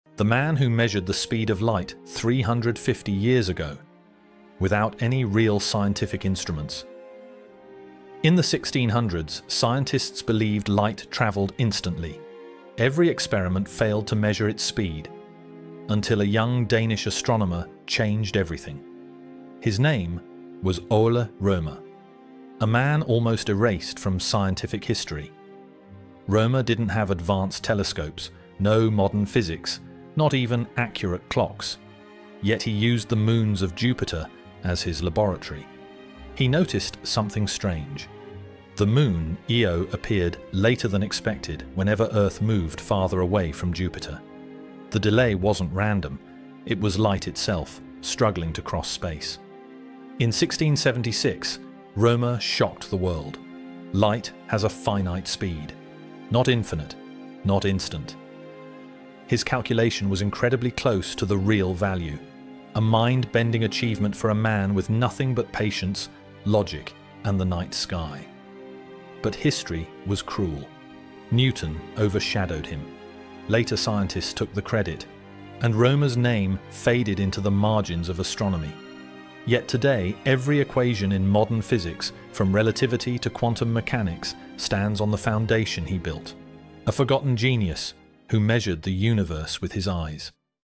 The Man Who Measured the Speed of Light… in 1676
A forgotten Danish astronomer who changed physics forever.
Ole Rømer measured the speed of light in 1676—using only Jupiter’s moons.
His discovery shaped modern science, yet history barely remembers his name.
From Memory – Forgotten Stories brings you the minds who shaped humanity… but were forgotten.